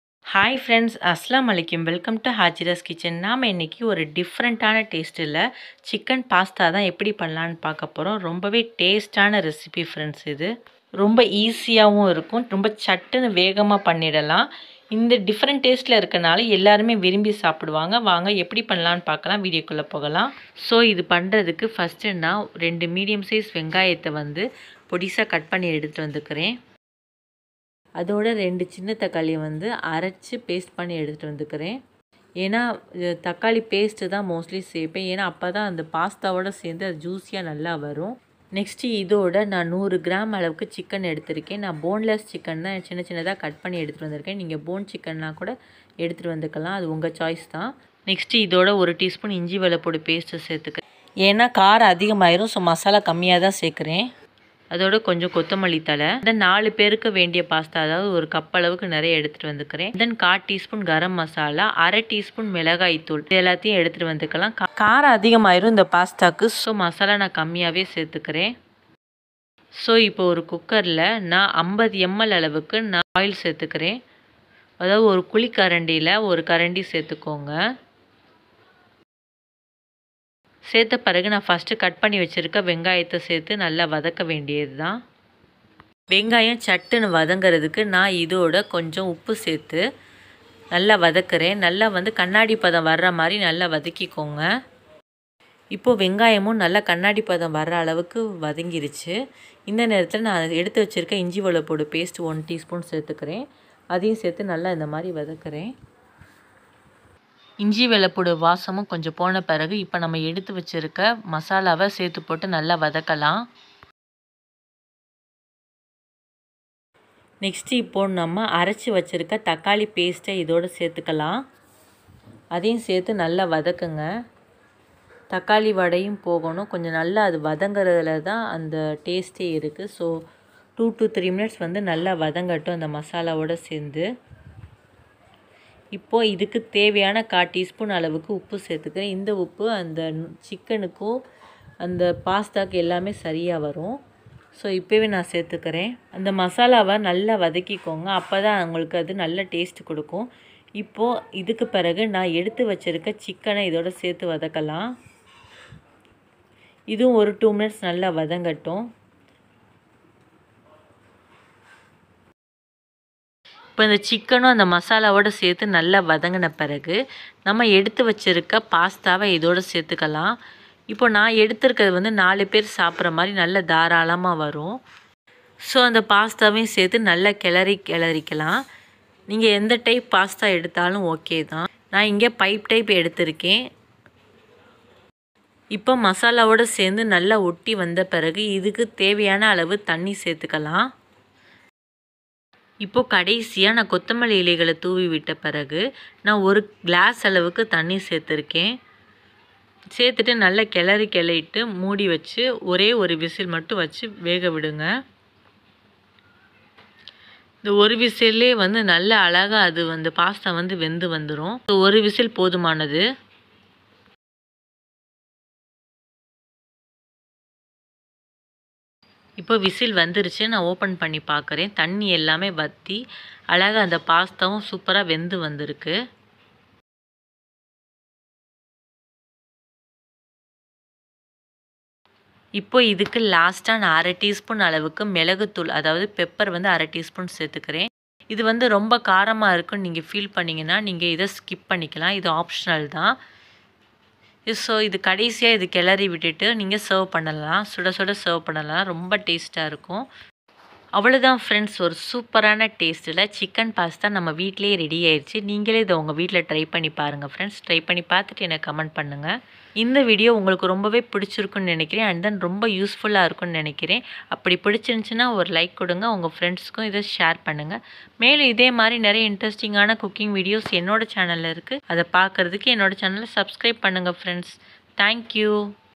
Tasty Chicken Pasta Recipe | Tiffin
Hai friends, In this video i am making the tasty and easy chicken pasta recipes. Indian style and quickly making recipe of chicken pasta i have made.
Chicken pasta is the delicious tiffen recipes and everyone to make by watching this video. Simple and easy ingredients i have used and take a 5 min of time. Follow my steps from this video you will get a delicious chicken pasta recipes.
Love Food and Enjoy the recipes.

Ingredients:

Pasta - 1 Bowl (Taken for 4 members)
Onion - 2 Med size onion (Smally chopped)
Chicken - 100 gm (boneless chicken)
Ginger & garlic paste - 1 tspn
Tomato - 2 (small size)
Chilli Powder - 1/2 tspn
Garam Masala -1/4 tspn
Coriander leaves